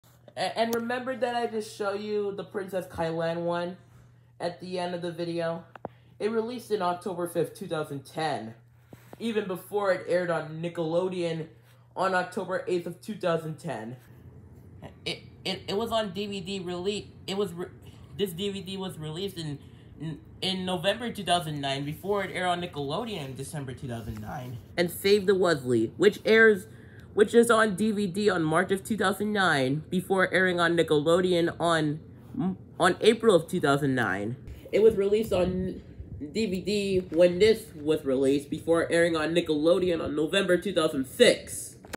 Me saying episodes were released on DVD before airing on Nickelodeon.
Here’s a montage of me saying episodes were released on DVD before airing on TV.